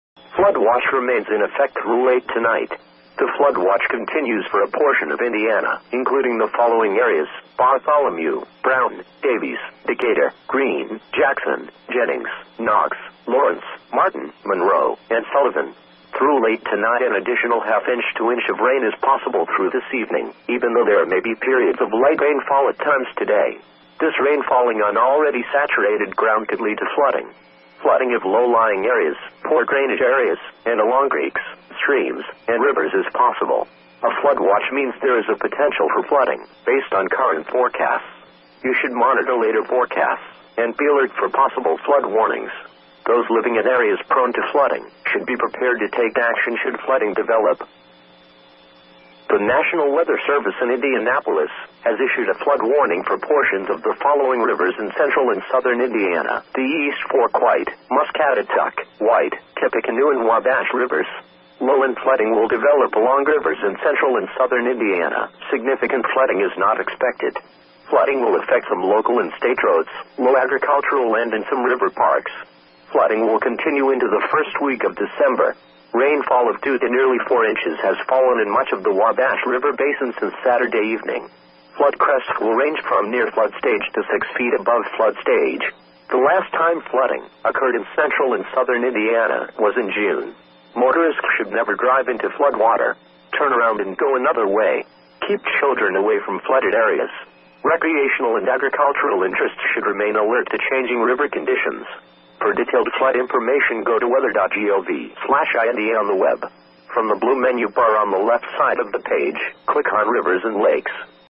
My First NOAA Radio Recording Attempt (Audio Only)
Thanks to WeatherUnderground, I located my local station tat does NOAA weather broadcasts and recorded the current advisory out.